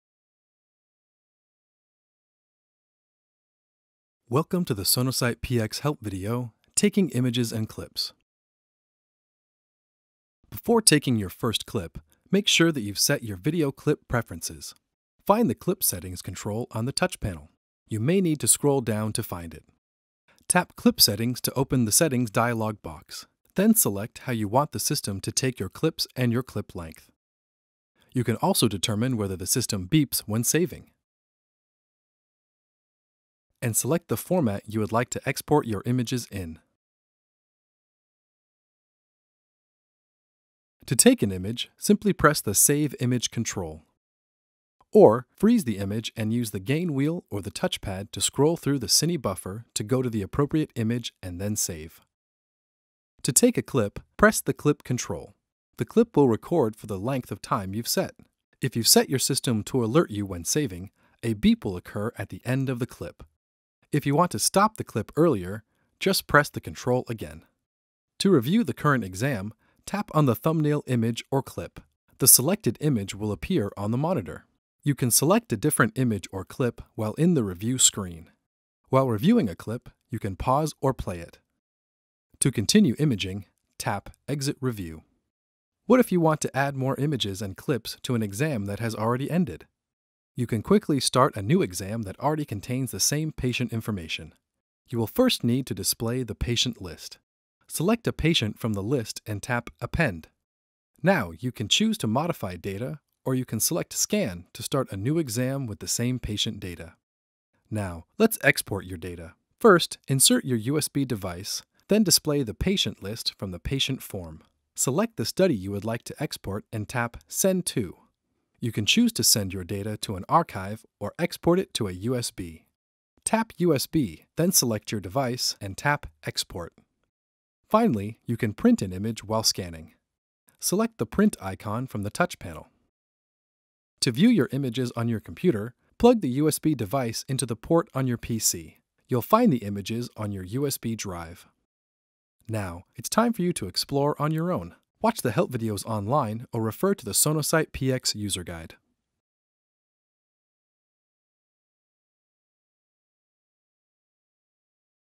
Sonosite PX Help Video - Taking Images and Clips
In this video, we will teach you how to take images and clips as well as how to customize this experience to your preference.